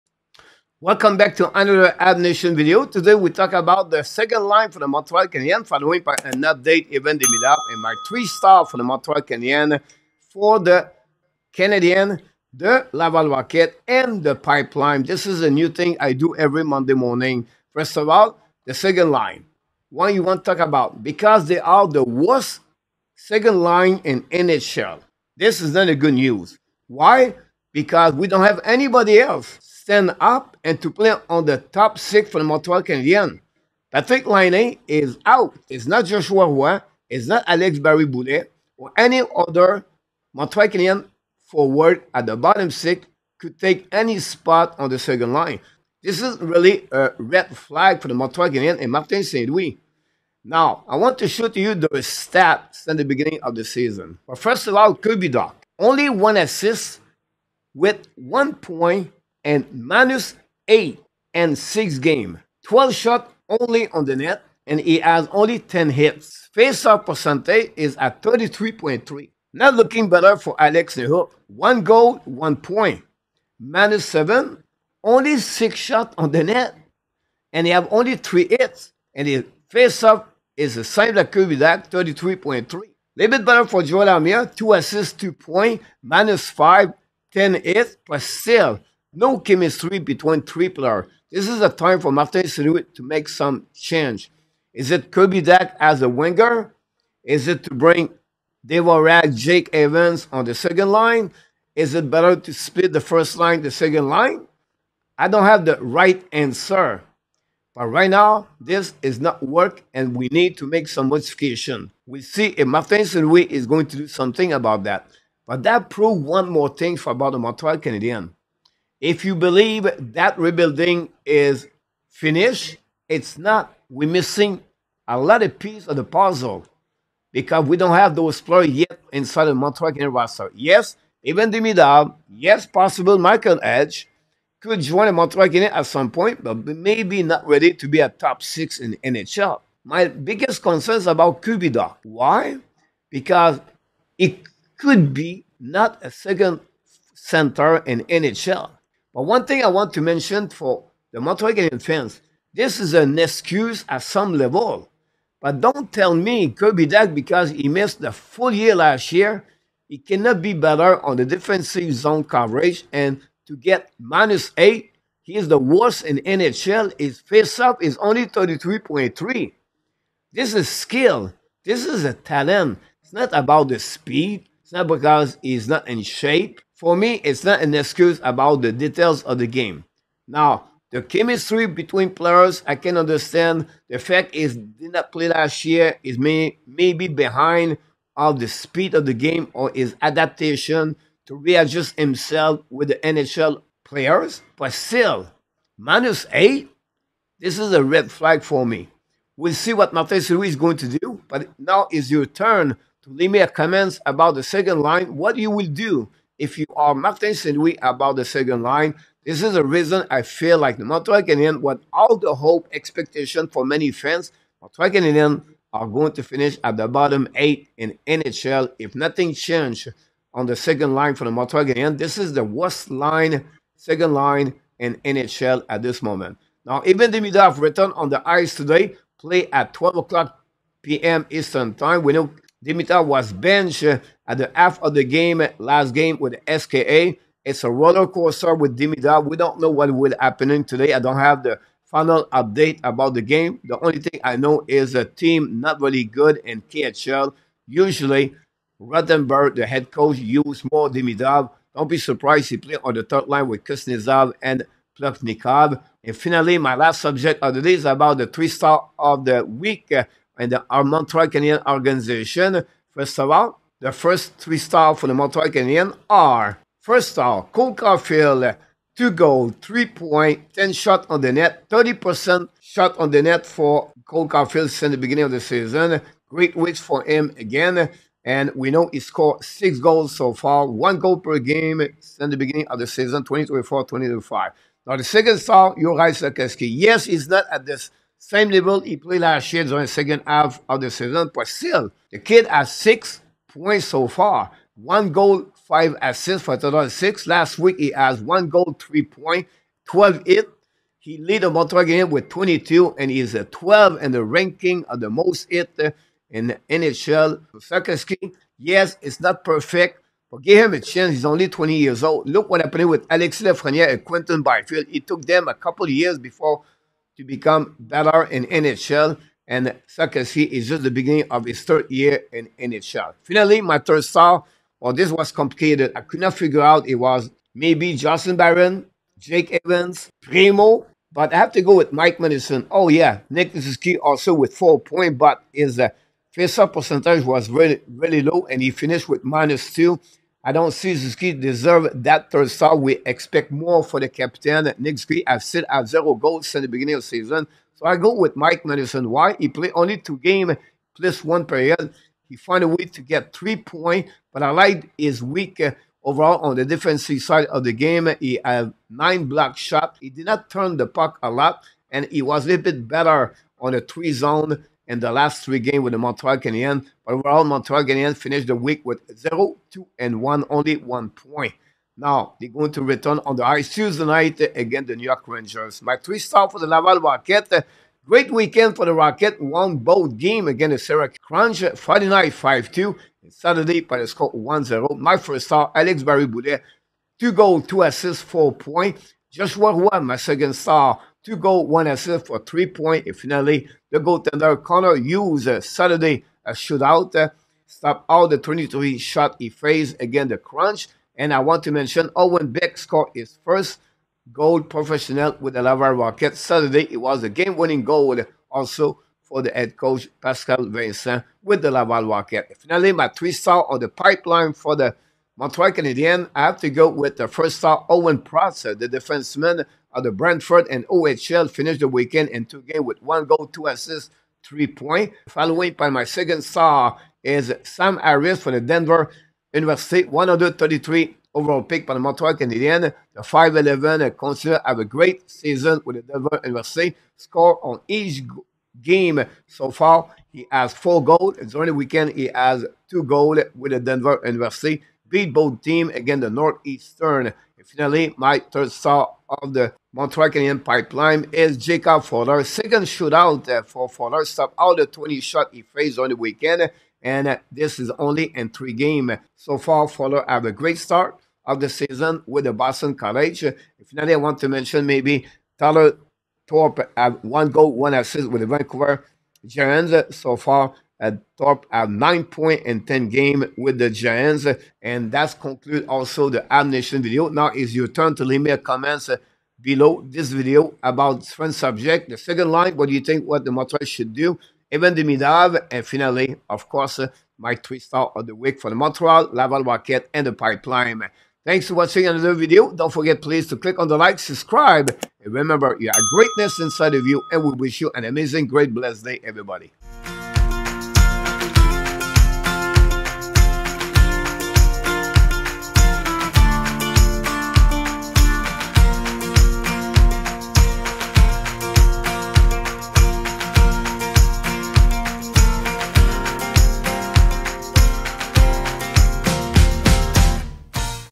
Time for Change: The Canadiens Need to Act Now
Habs Nation Video of the Day! 🏒 Today, Coach breaks down the struggles of the second line, focusing on key players like Kirby Dach, Joel Armia, and Alex Newhook, and how their performance is impacting the team.

Plus, we’ve got exciting updates on Ivan Demidov and our 3 Stars of the Week!